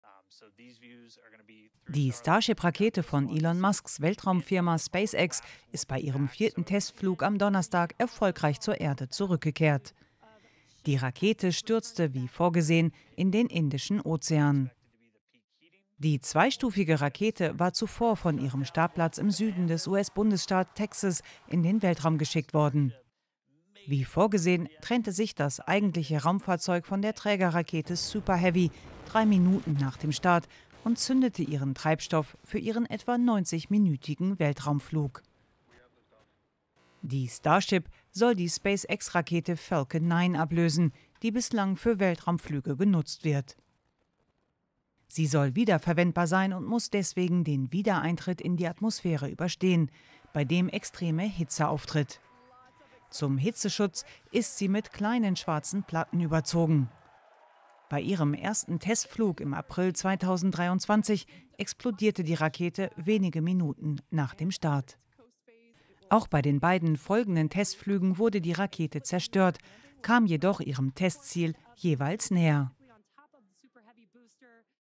Elon Musks Weltraumfirma: SpaceX-Rakete „Starship“ kehrt heil zur Erde zurück
Die Rakete „Starship“ von Elon Musks Weltraumfirma SpaceX kehrt beim vierten Testflug erstmals zur Erde zurück. Bei ihrem ersten Flug im April 2023 ist die Rakete wenige Minuten nach dem Start explodiert.